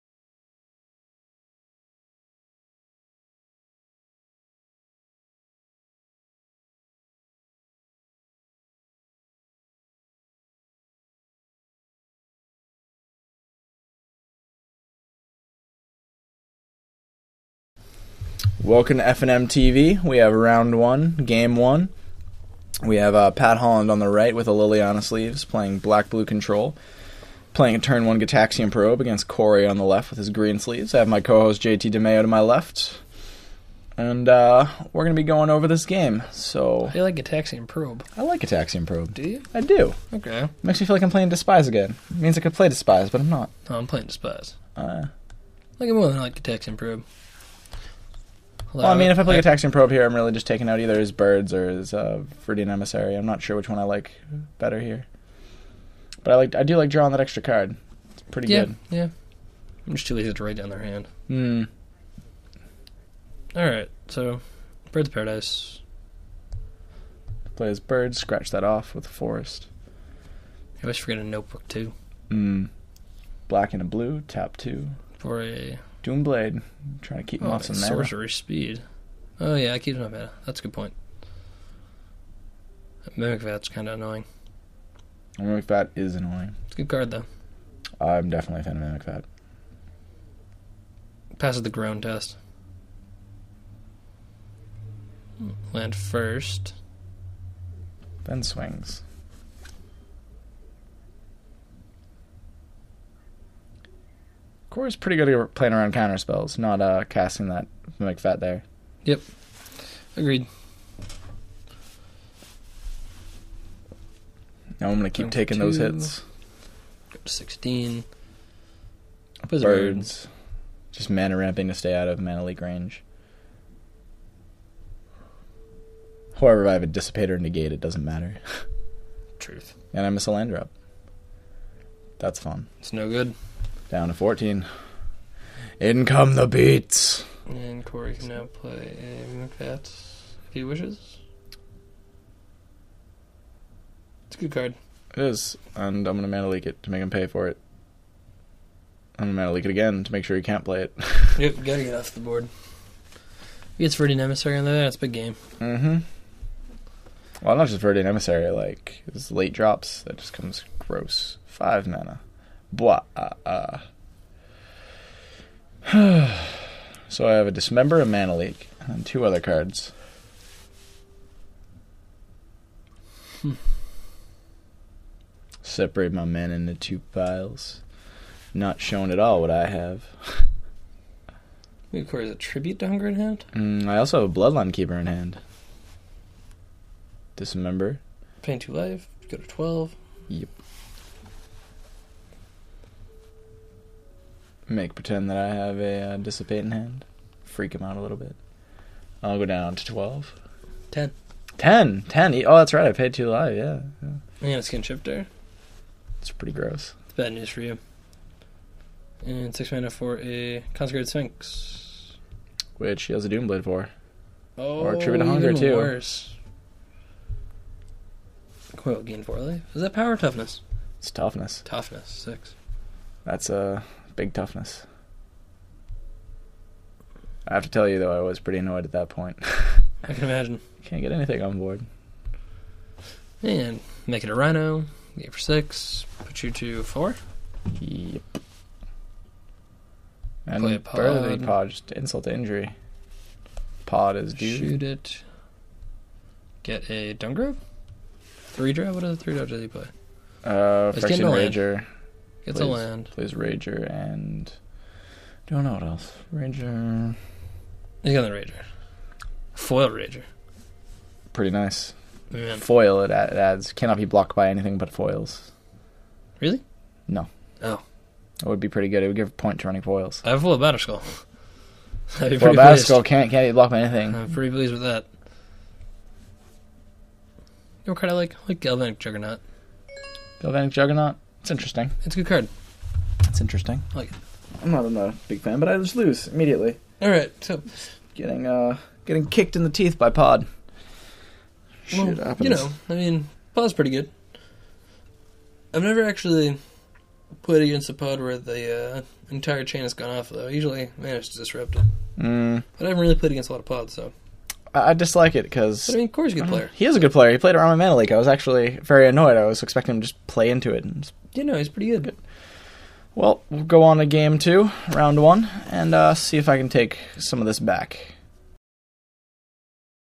Harrison's FNM TV 11/04/11 - Round 1 Game 1 // BG Pod vs UB Control
FNM TV // Round 1 Game 1 // Corey Patient vs Patrick Holland // BG Pod vs UB Control // 0-0

THE RETURN OF JT DIMAIO :O